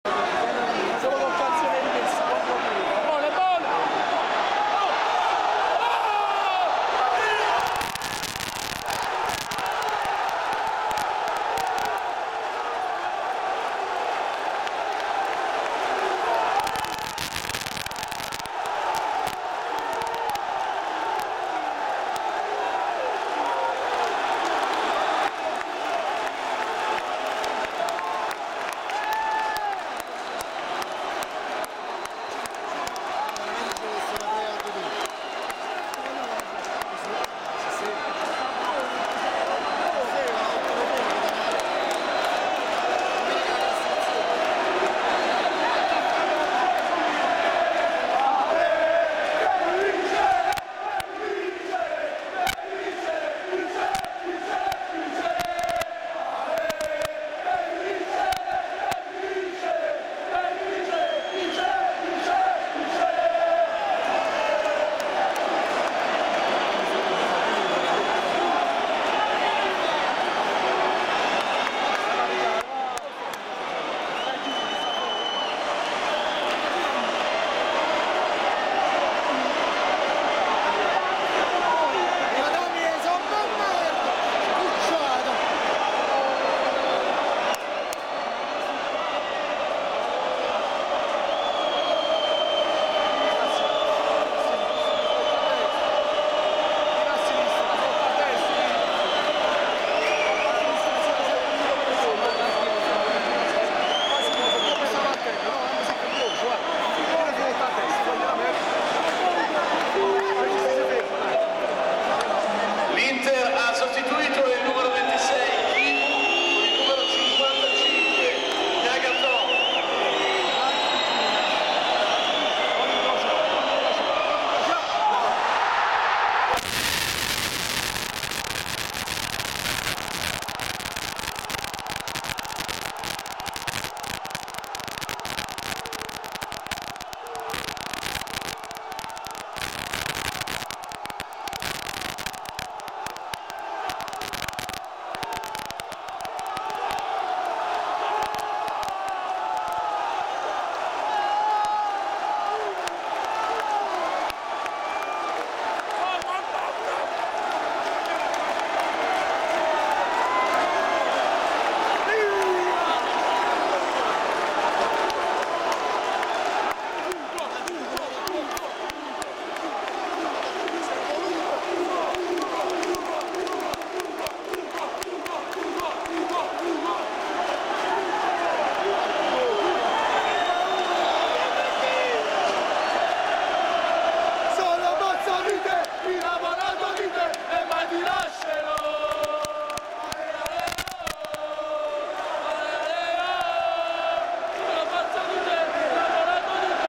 Inter-Napoli 0-3 01/10/2011 Gol Live di Campagnaro dal Settore ospiti in HD
Il fallo di rigore , lo sbaglio dal dischetto di Hamsik e la successiva rete di Hugo Campagnaro Live dal settore ospiti in HD